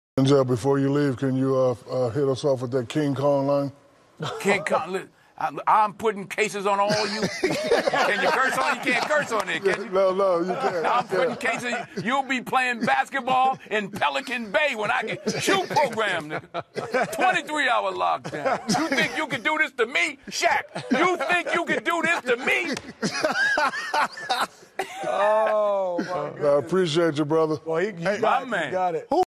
Shaq Asks Denzel To Recite King Kong Line
Shaq ask Denzel To Recite Infamous Lines from Training day